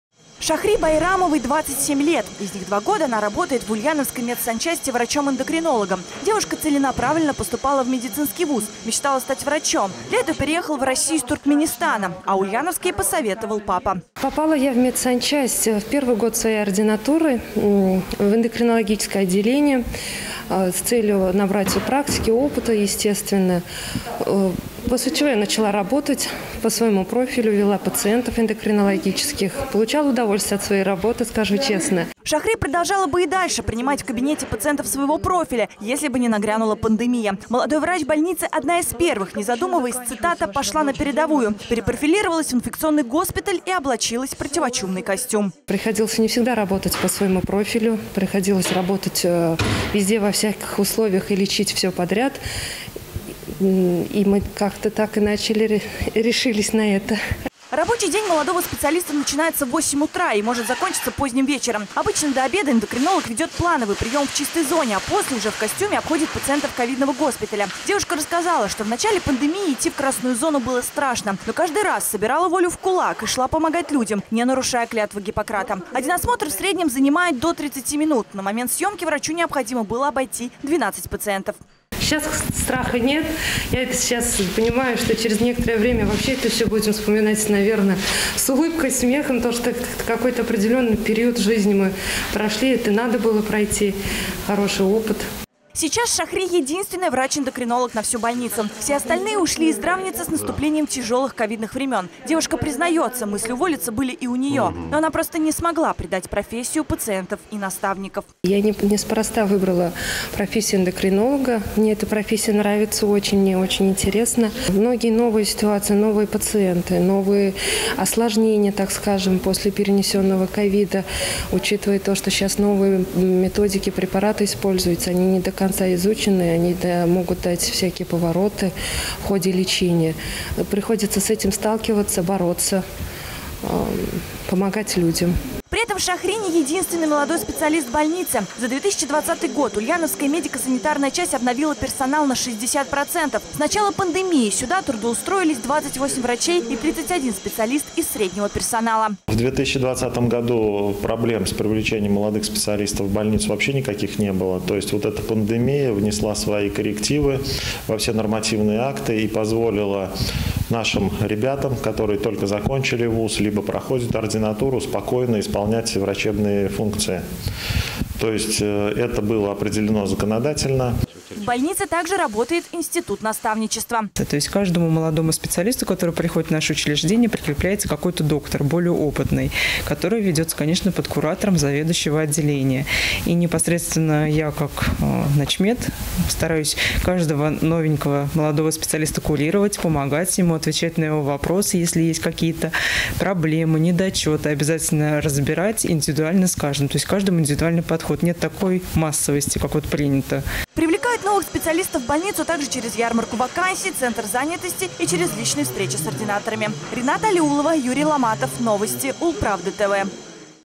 Дорогу молодым. Больницы Ульяновской области пополняются новыми врачами
По долгу службы и по зову сердца. Свою профессию героиня нашего следующего сюжета выбрала от большой любви к людям. В своей семье - она первая, кто  стал врачом. Молодая и амбициозная 27-летняя девушка обсуживает своих пациентов планово, работает в красной зоне и не боится трудностей.